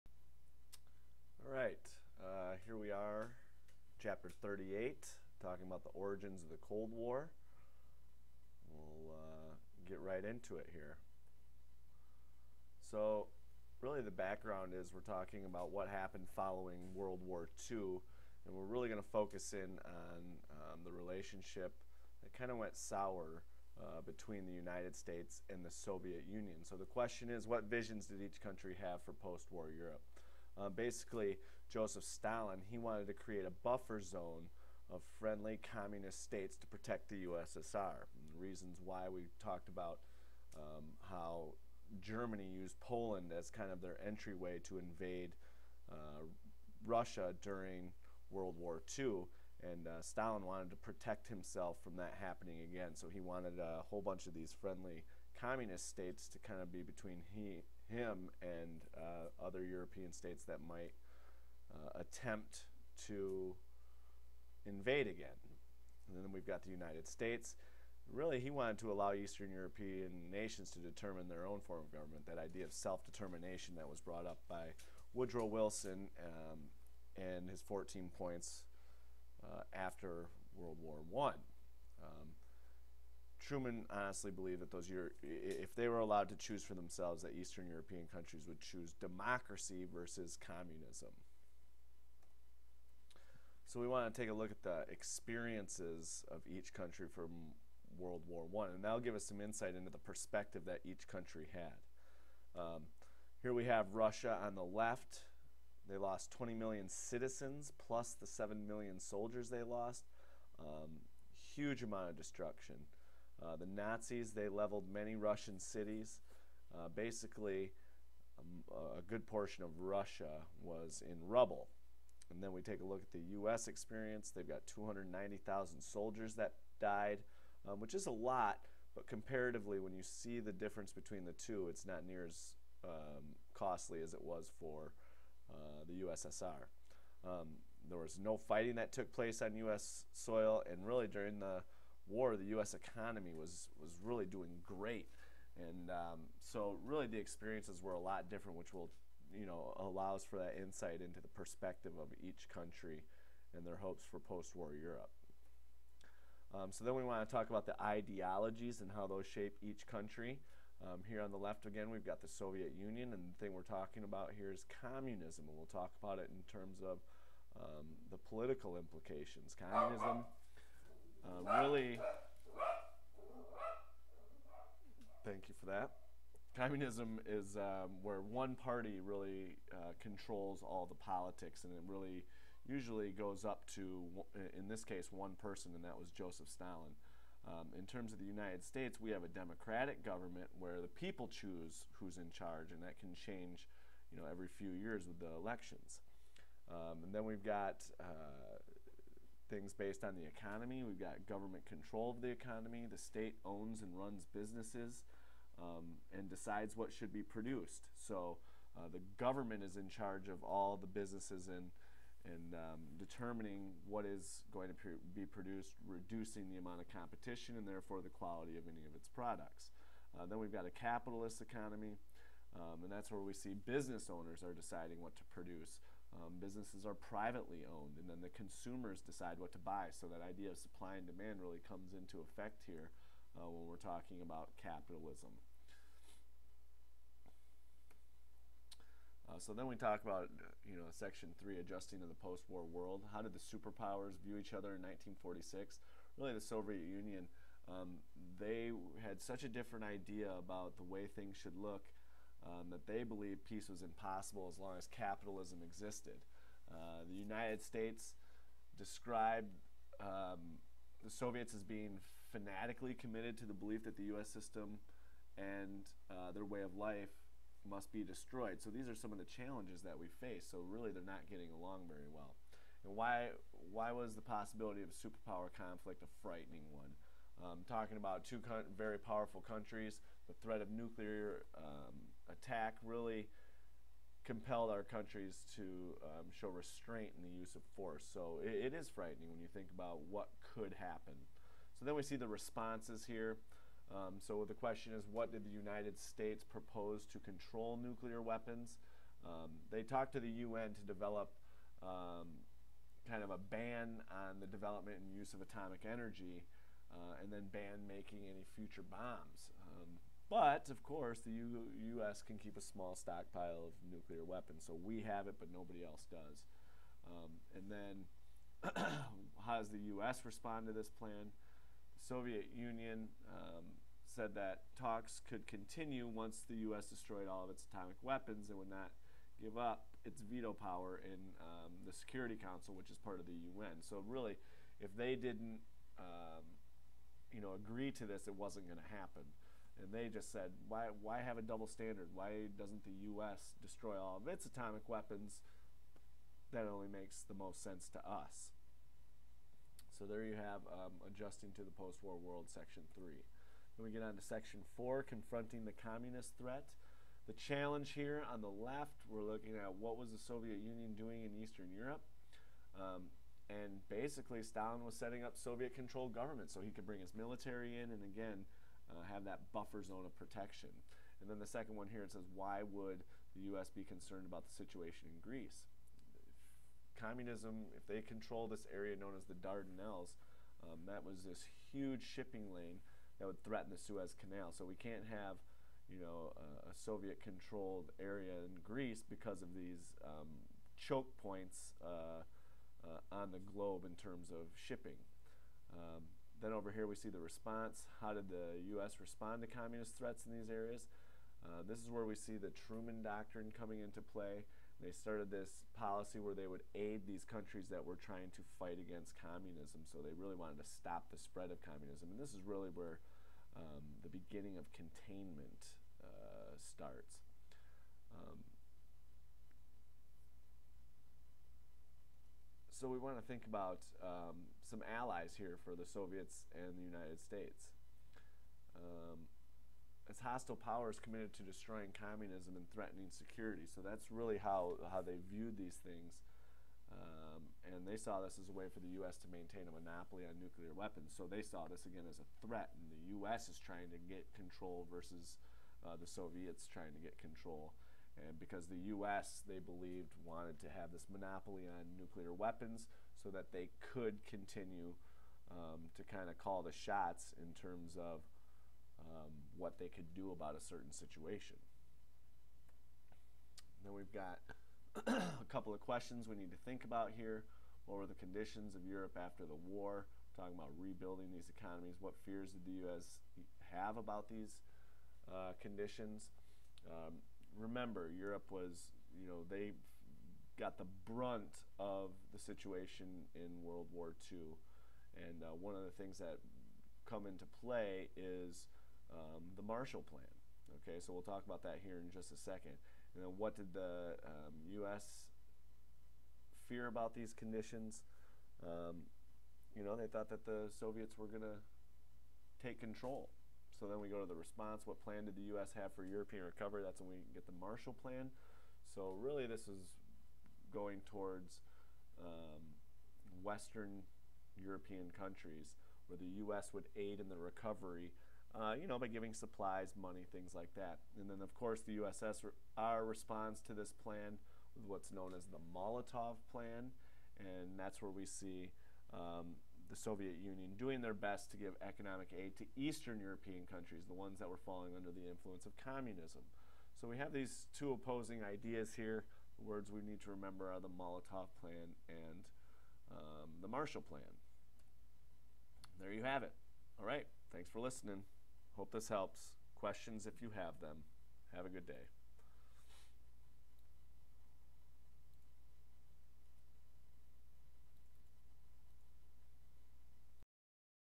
The Origins of the Cold War
TCI History Alive!: Pursuing American Ideals
Chapter 38: The Origins of the Cold War
Yalta Conference
Potsdam Conference
UN Atomic Energy Commission
Truman Doctrine
Molotov Plan
Marshall Plan
Iron Curtain